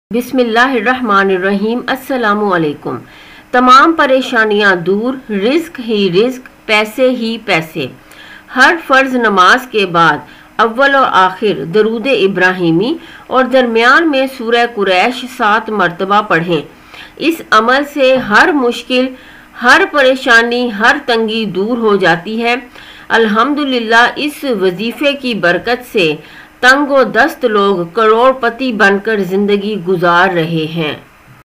Wazifa Of Surah QURAISH to Increase Money | Ubqari Wazaif
In this video wazifa of surah Quraish is explained to increase money.